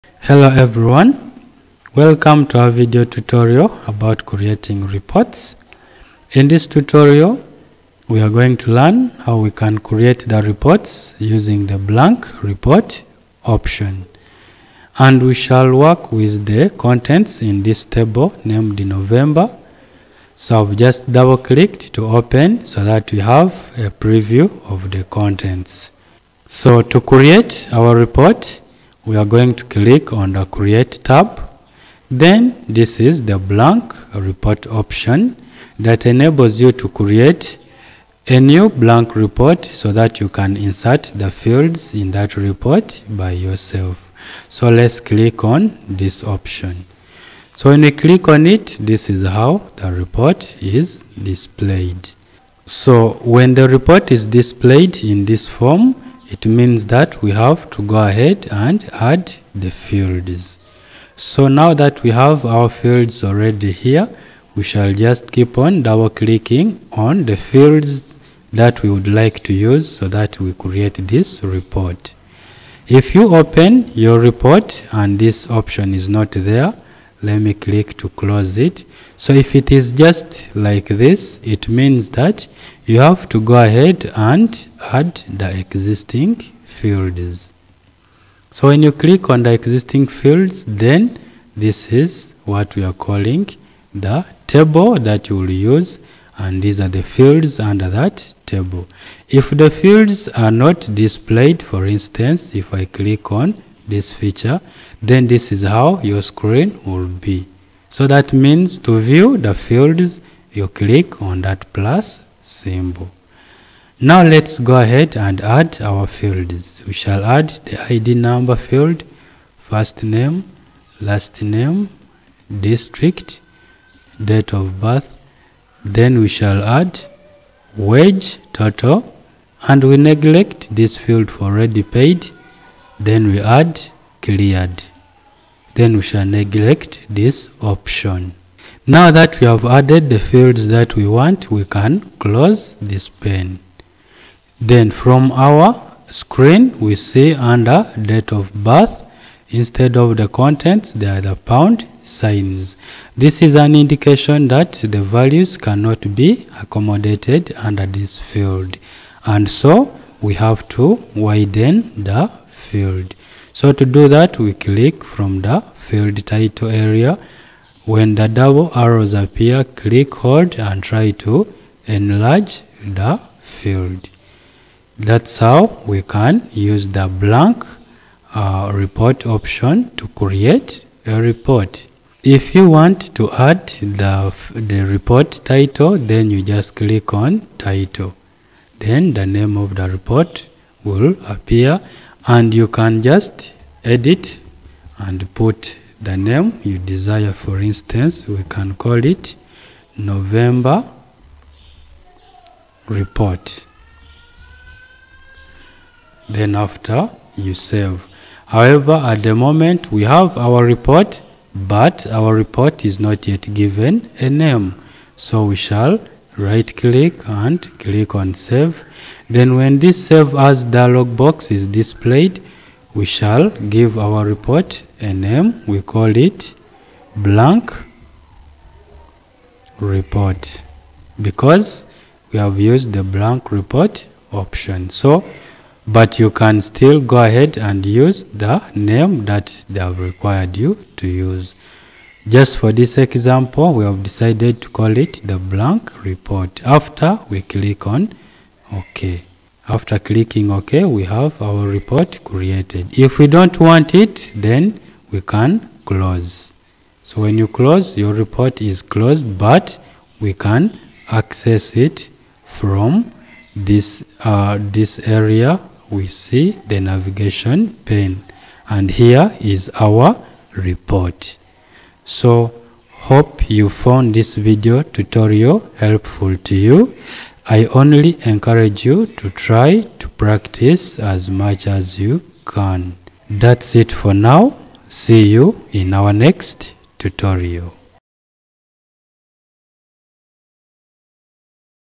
Creating reports from the Blank report option
This tutorial is about Creating reports from the Blank report option